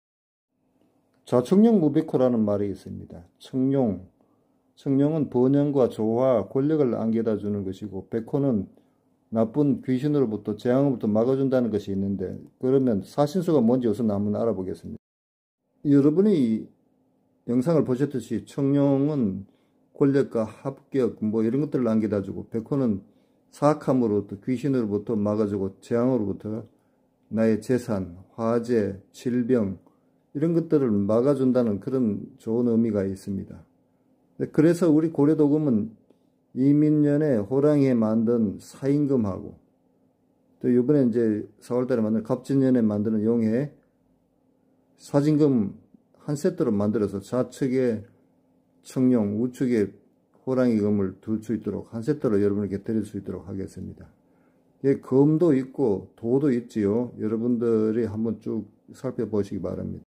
청룡 과 백호 도검 도검 고려도검 진검 벽사의검 사인검
좌 청룡 우 백호 안에 있어면 모든게 평안히고 잘된다는 풍수지리 설이 있습니다.   이 를믿는 분 들을 위해 입인년 호랑이 해 만든검이나 도 갑진년 용의 해 만드는 검 과 도 를 사무실이나 서재 응접실에 비치해보면 어떨까요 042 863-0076, 0078